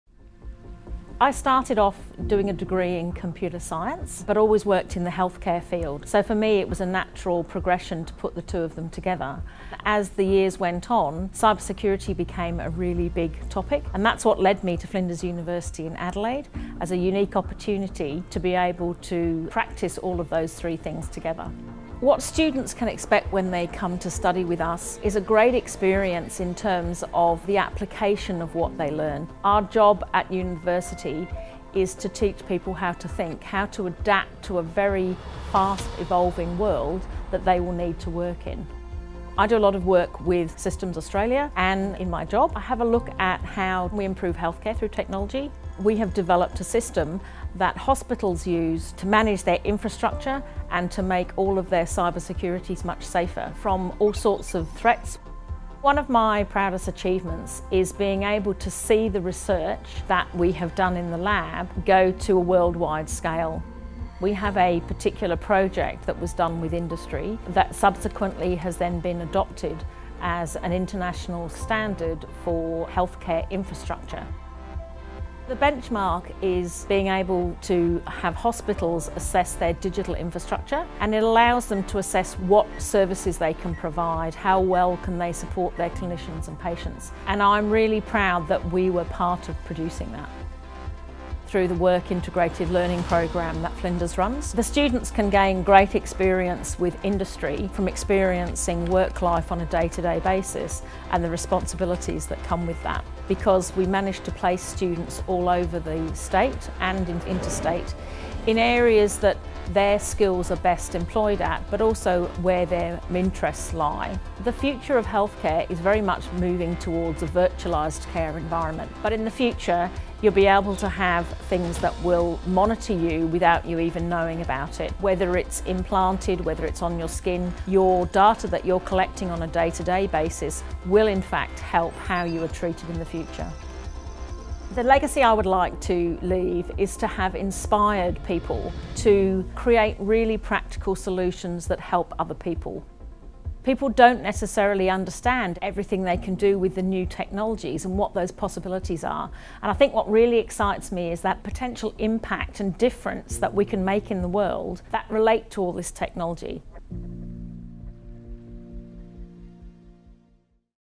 Study Flinders Computer Science
Meet Professor Trish Williams – Trish is a Flinders educator in computer science, cybersecurity and expert in Digital Health Systems at Flinders.

“I started off doing a degree in Computer Science but always worked in the healthcare field, so for me it was a natural progression to put the two of them together. As the years went on cybersecurity became a really big topic and that is what led me to Flinders University in Adelaide, as a unique opportunity to practice all of those three things together.” Professor Trish Williams

"What students can expect when they come to study with us is a great experience in terms of the application of what they learn." Professor Trish Williams